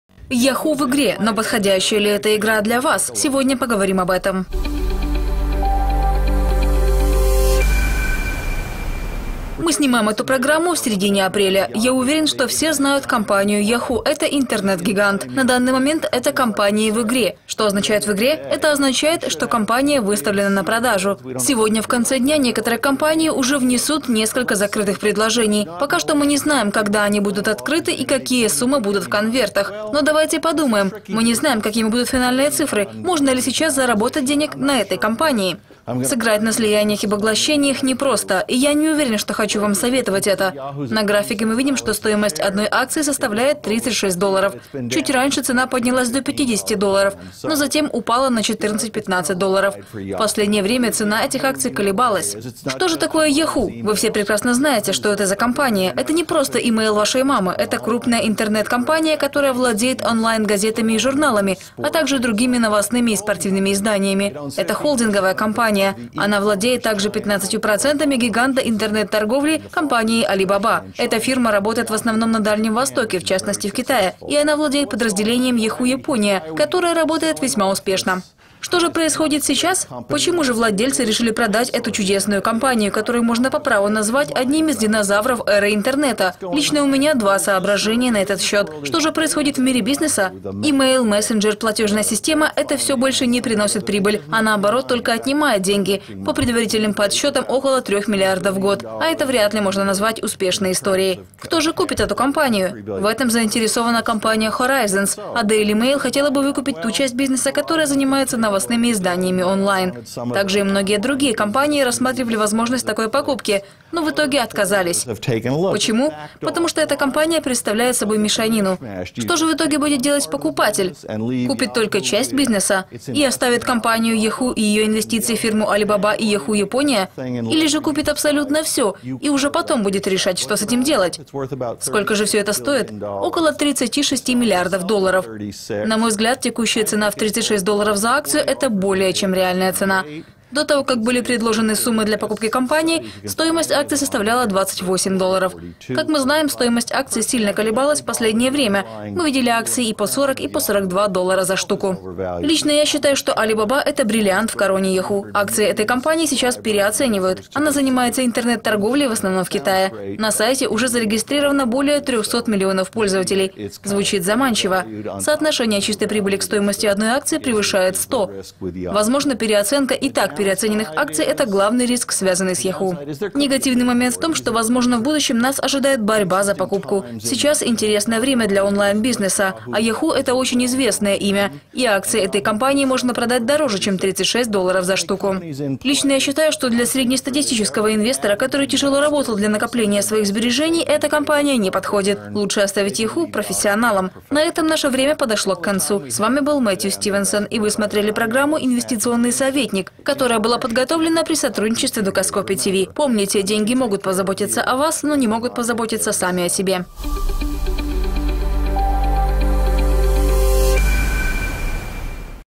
Инвестиции в Yahoo!
Стоит ли покупать акции компании Yahoo? Мэттью Стивенсон.